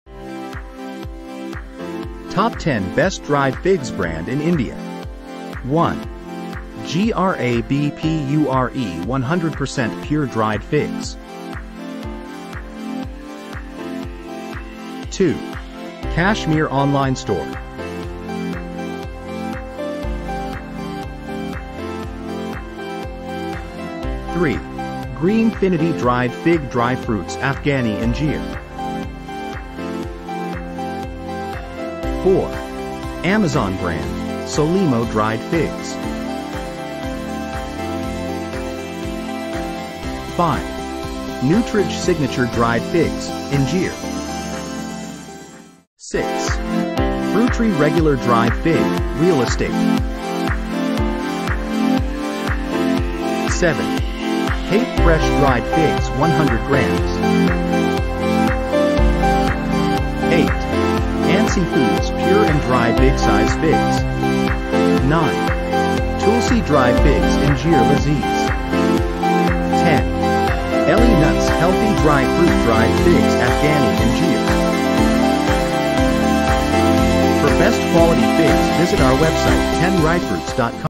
Top 10 Best Dried Figs Brands In India || Fig Benefits In Pregnancy
"Experience the sweetness of quality with our guide to the Top 10 Dried Figs Brands in India! 🌿🇮🇳 Explore exquisite selections that redefine taste and nutrition. From traditional favorites to innovative blends, discover the perfect dried figs to elevate your snacking experience. Watch now and indulge in the rich flavor and health benefits these top brands bring to your table! 🌟✨ DriedFigsBrandsIndia FigDelights"
This Video Is A Compilation Of The Best Brands Of Dried Figs Available In The Market Today. If You Are Looking For The Best-Dried Fig Brand In India, Then You Have Come To The Right Place. We Have Compiled A List Of The Top 10 Best-Dried Fig Brands In India. We Have Also Provided A Short Description Of Each One Of These Brands. So, Let’s Get Started.

Links to all the brands:
1. GRABPURE 100% Pure Dried Figs
Check Price On Amazon

3.GreenFinity Dried Fig Dry Fruits Afghani Anjeer,

4. Amazon Brand - Solimo Dried figs,

5. Nutraj Signature Dried Figs (Anjeer)

6. Fruitri Regular Dried Fig,

7. Cape Fresh Dried Figs 100g,

8. Ancy Foods Pure and Dry Big Size Figs,

9. Tulsi Dry Figs Anjeer Lazeez,

10. Elly Nuts Healthy Dry Fruit Dried Figs Afghani Anjeer

Hope you found the best brand for yourself.